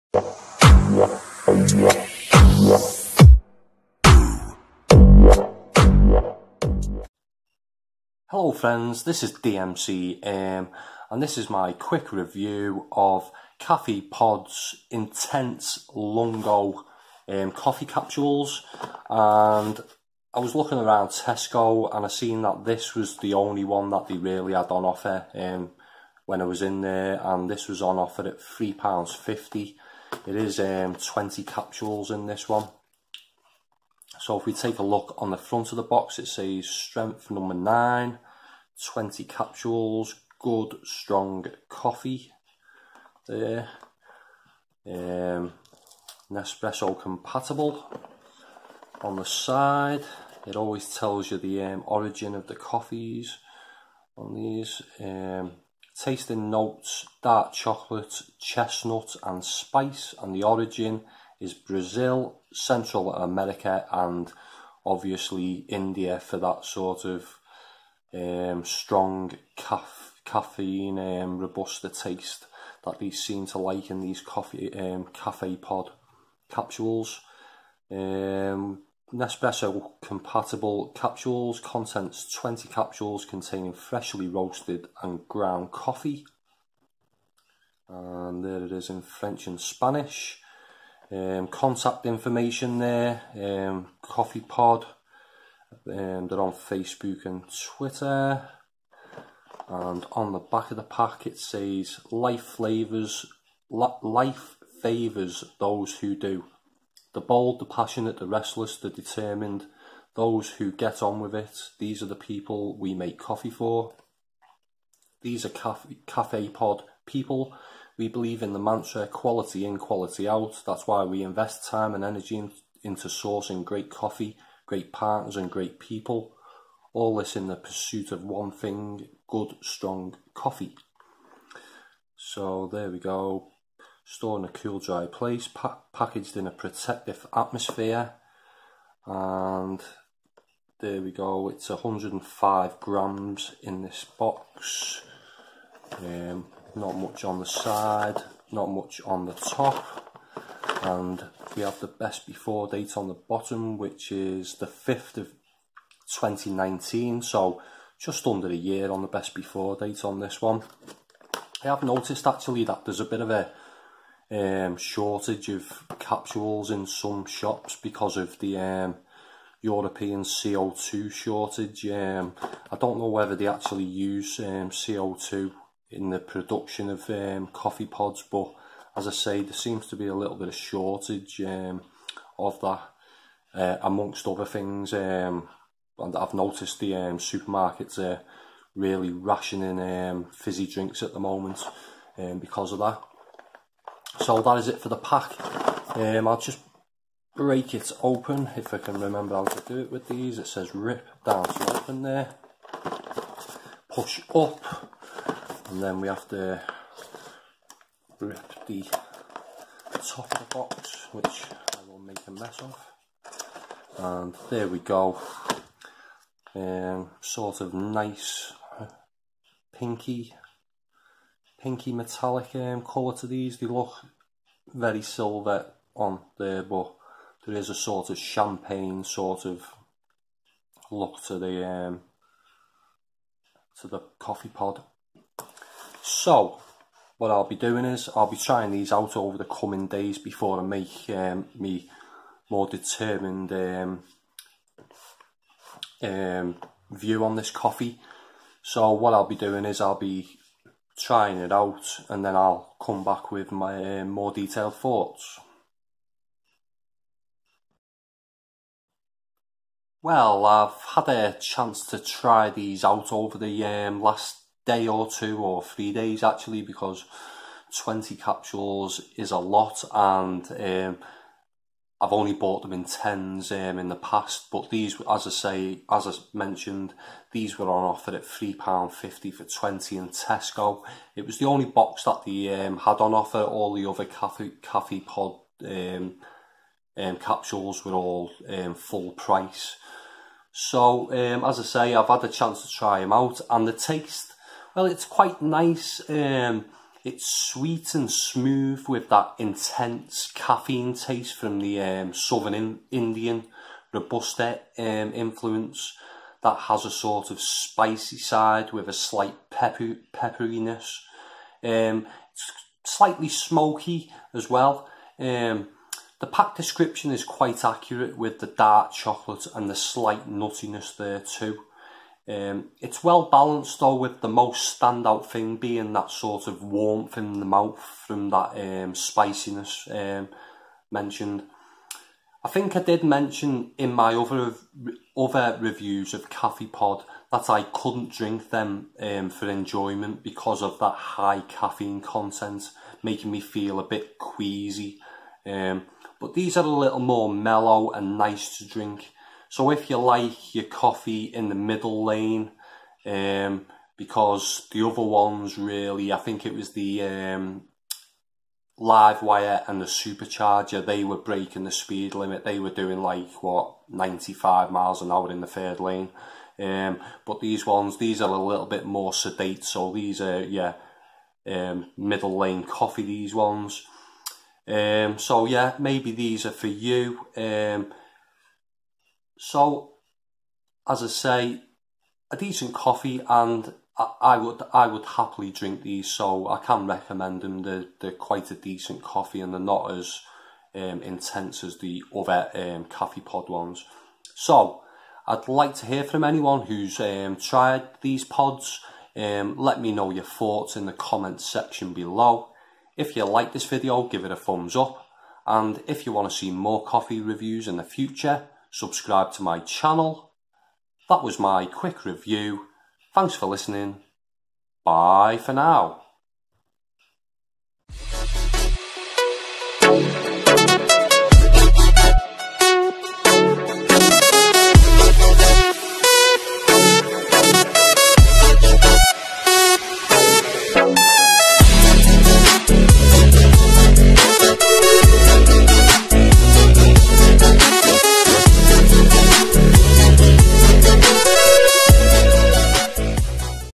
Cafepod Intense Lungo Coffee Pod's Review.
This is my quick review of Cafepod's Intense lungo coffee capsules...a nice intense coffee with a warmth from the caffeine high content..